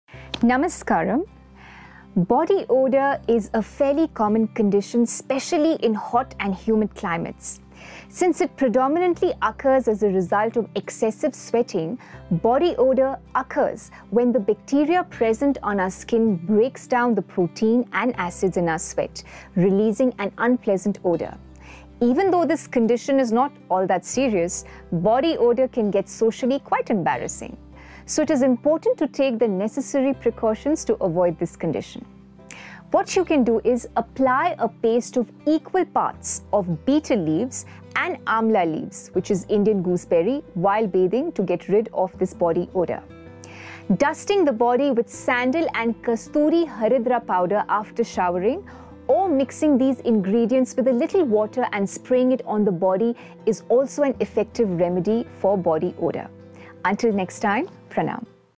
How To Remove Body Odor Naturally | 100 % Results
In This Video You're Going To Learn About,How To Remove Body Odor(Don't Skip 😀‼️)

Namaskaram,
Body Odor Is A fairly common condition, especially in hot and humid climates since it's predominantly occurs us a result of excessive sweating.

Body odor occurs when the bacteria present on our skins breaks down the protein and acids in our sweats, realising an unpleasant odor. Even though is condition not all serious, body odor can get socially quite embarrassing so it is important to take the nesscery precautions to avoid this condition.

What you can do is apply paste of equal parts of betel leaves and amla leaves which is indian gooseberry while bathing.

To get rid of this body odor, dusting the body with sandal and kasturi daridra powder after showering or mixing this ingredients with a little Water and spraying it on the body it is also effective for a body odor.

We'll See You Soon, Pranam !!

➡️➡️To receive Yogic Food Tips, Lifestyle Tips, Immunity Boosting Practices, Program Updates, Sadhguru Live , join us on: